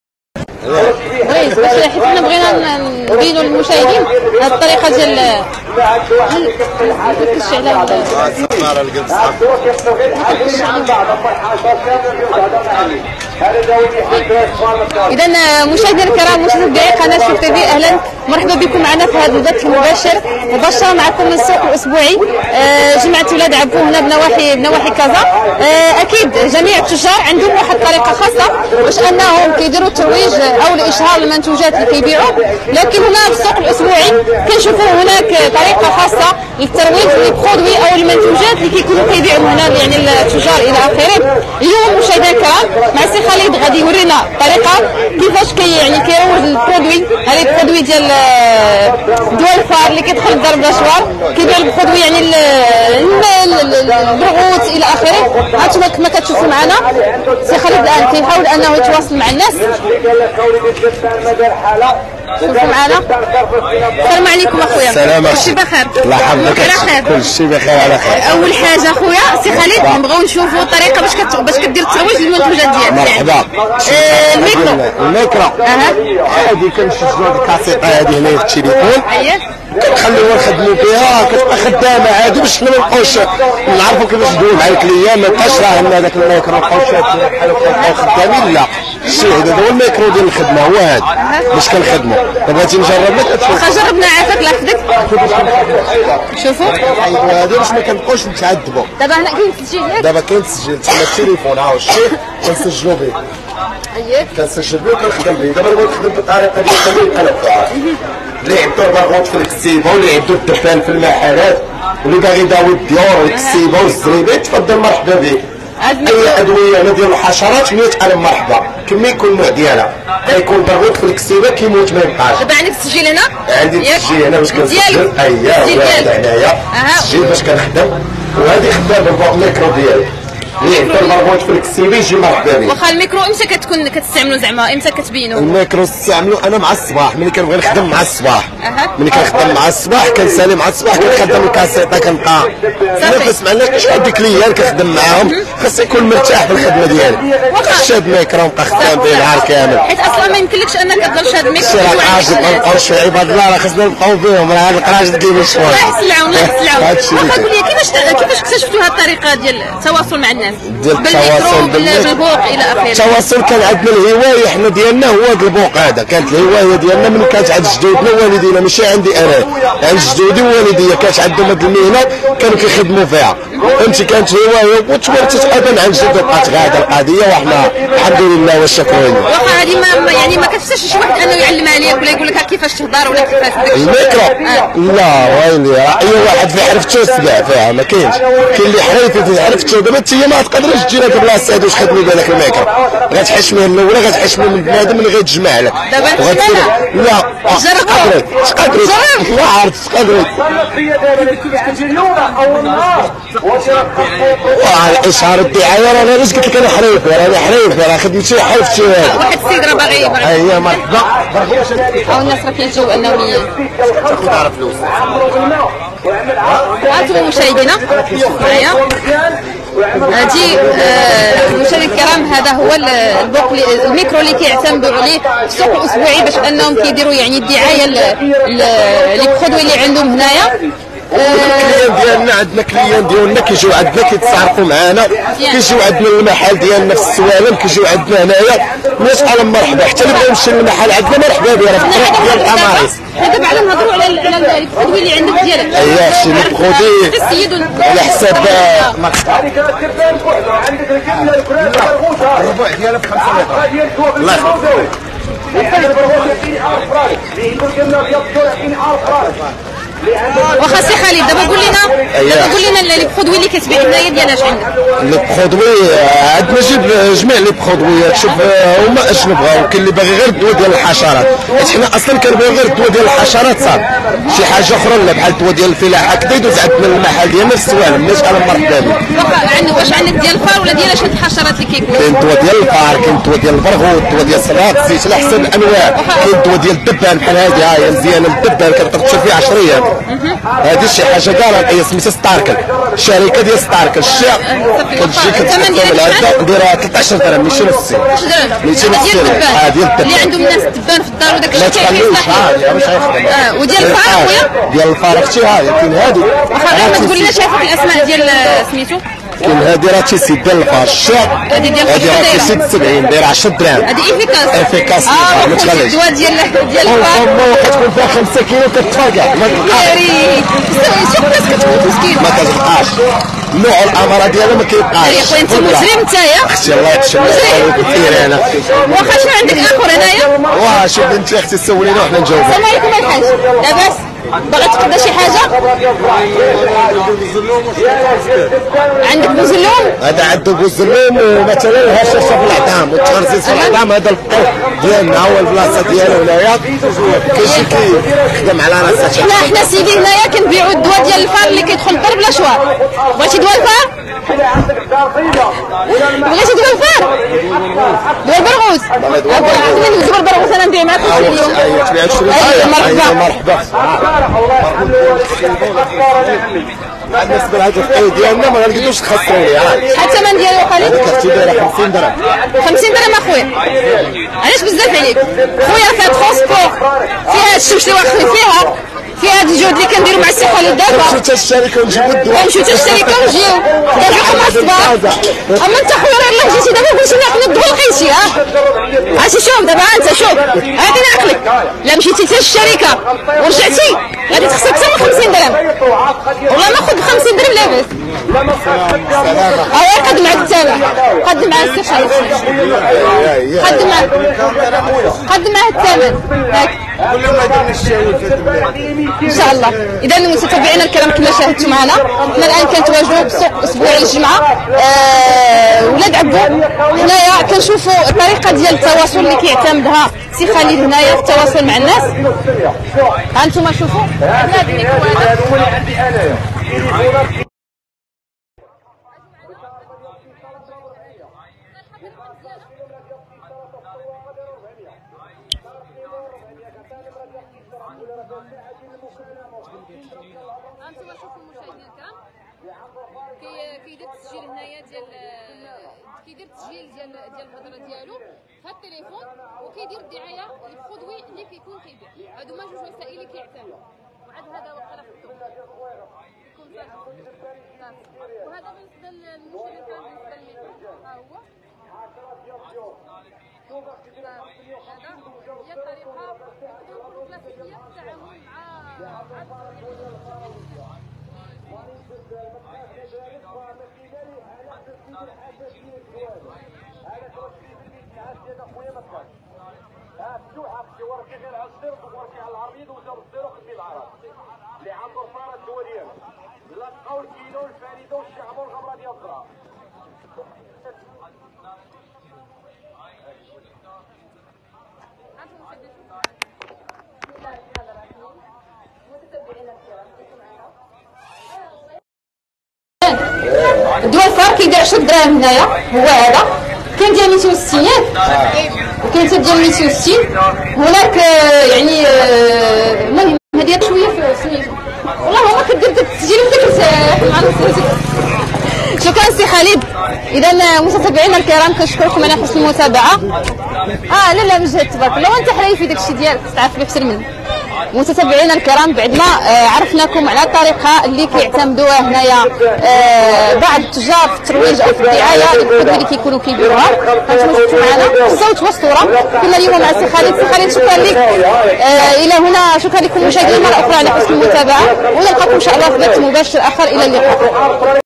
بائع مبيدات الحشرات عندو طريقة خاصة للماركوتينغ و مبدع عبارة"دوا الفار لي تيدخل الدار بلا الشوار"
عاجل وعلى المباشر: خالد بائع مبيدات القوارض و الحشرات عندو طريقة خاصة للماركوتينغ و مبدع عبارة"دوا الفار لي تيدخل الدار بلا الشوار" غدي يكشف لينا على أحسن ماكين في مبيدات القوارض